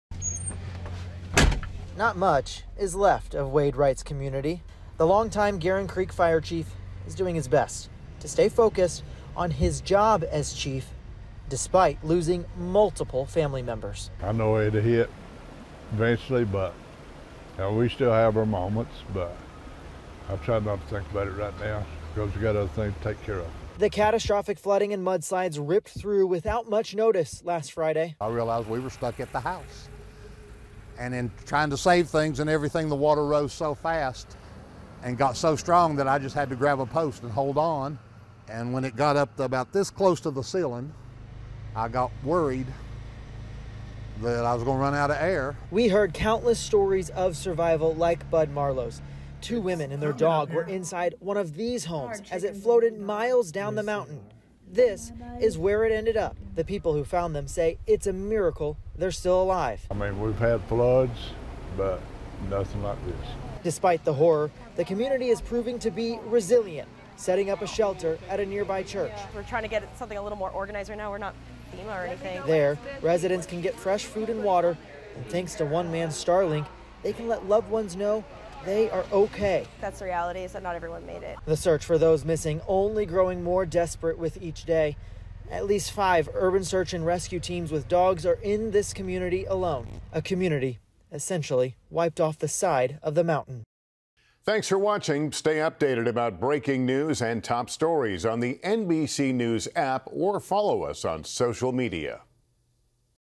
'We’ve had floods, but nothing like this': Garren Creek, N.C., devastated by Helene flooding
The tiny North Carolina community of Garren Creek is dealing with devastating losses after nearly wiped away by Hurricane Helene's floodwaters. WRAL's Brett Knese reports.